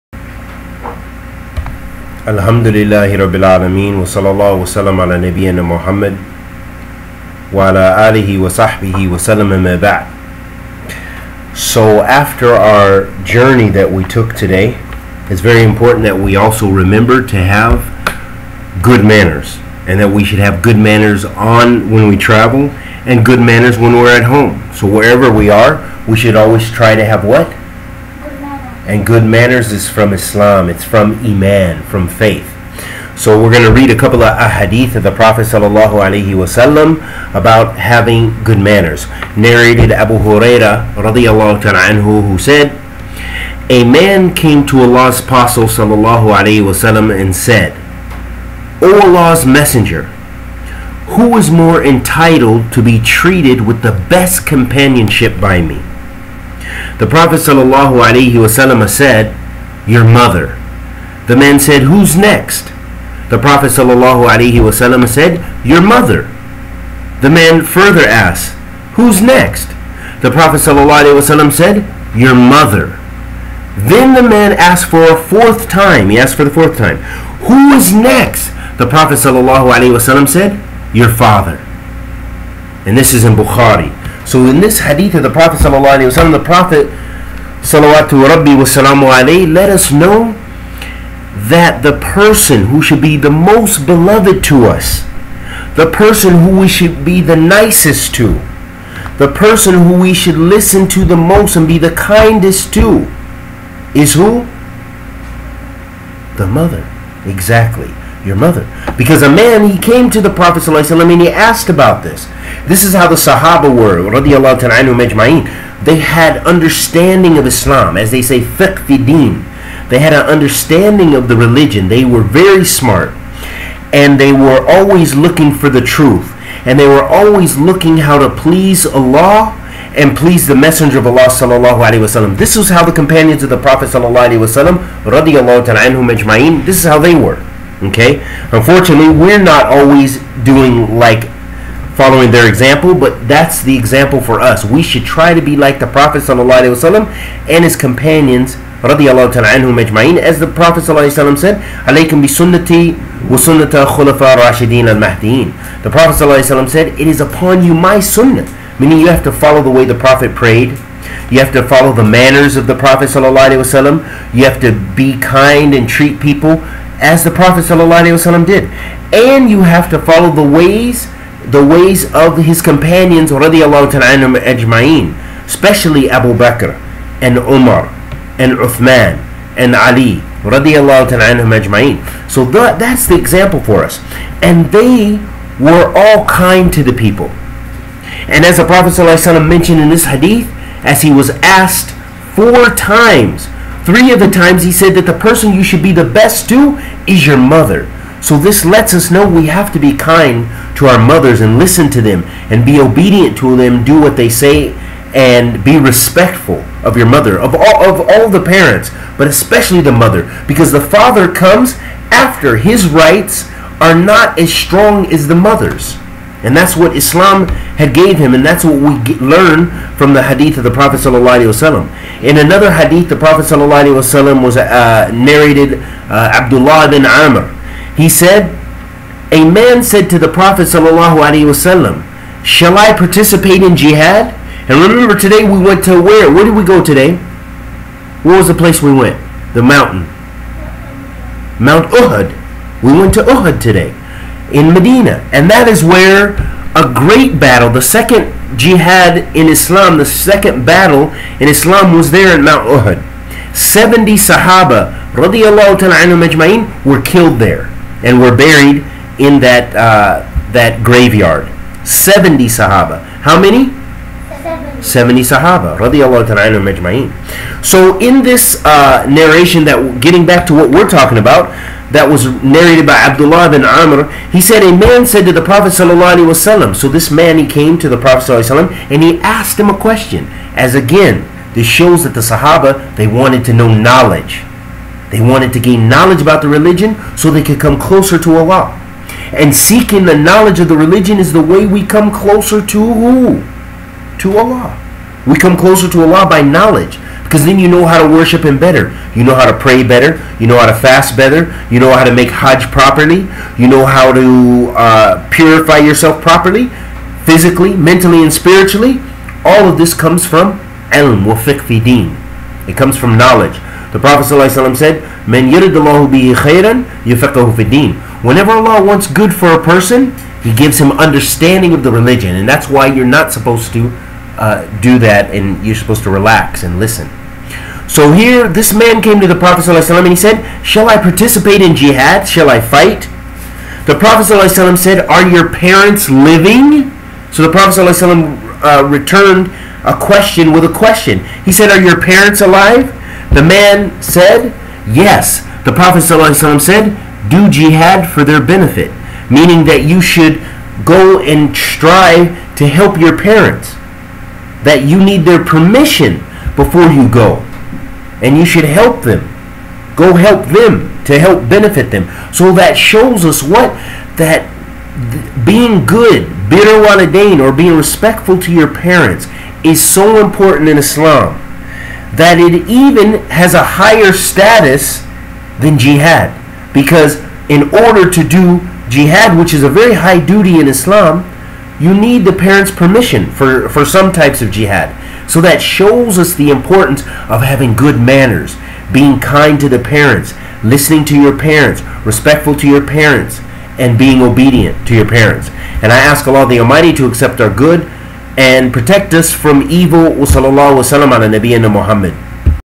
Please Listen To Your Parents And Be Kind O Muslims!
Advice for all especially children